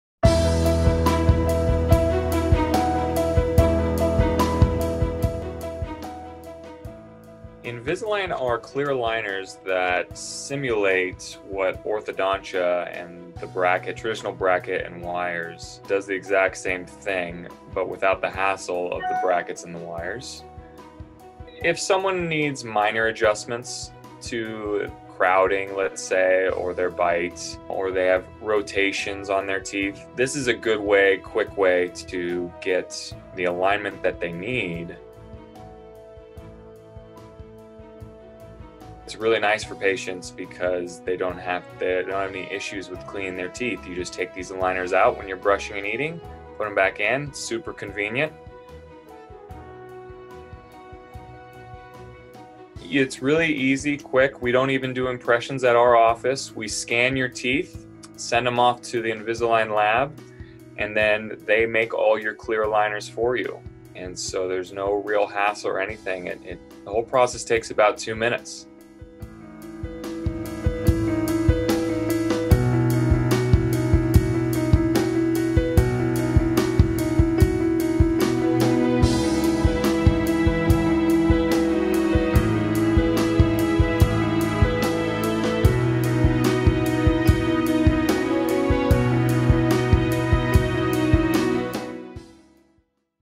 All you need to know about Invisalign at Volterra Dental explained by Dr. Richard Blackburn
Do you have crooked teeth or crowded teeth? Are you someone who is tired of traditional metal braces and looking for something more comfortable to make your smile straight? Then Invisalign is for you. Watch this video as Dr. Richard Blackburn from Volterra Dental explains all you need to know about Invisalign.